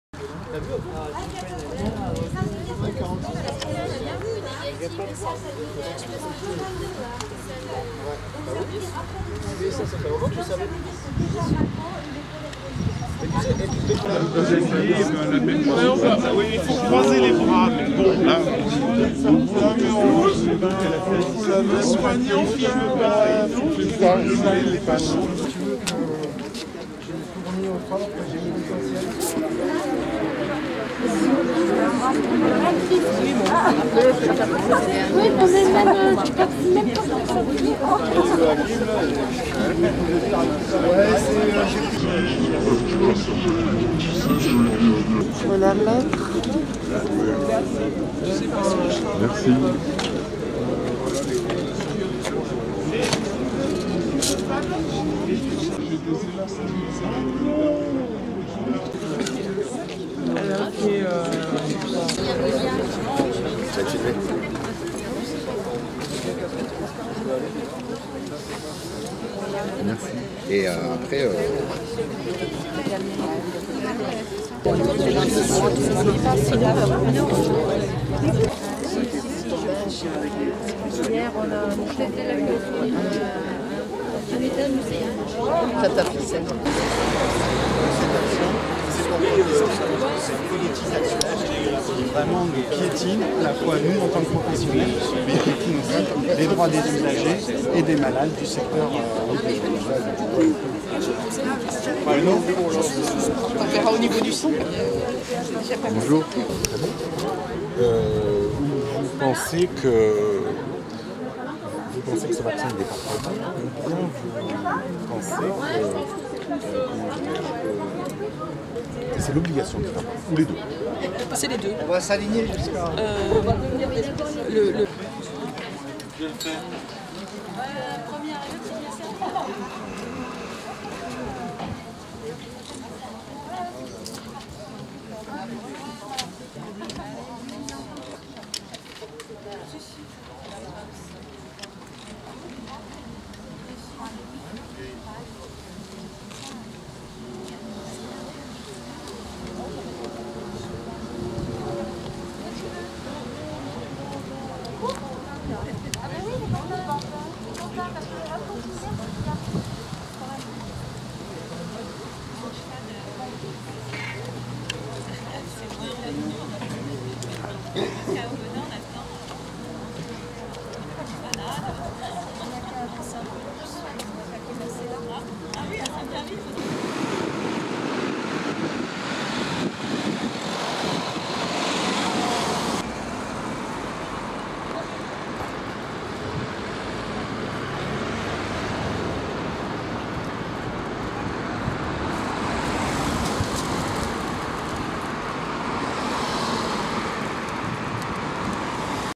pole emploi Aubenas 30/9
action devant le pole emploi avec /et en solidarité avec un nombre important de personnel soignant et de santé suspendus. La presse était là du moins le dobé et France bleue ; Fr3 s'était engagée à participer mais s'est illustrée par son absence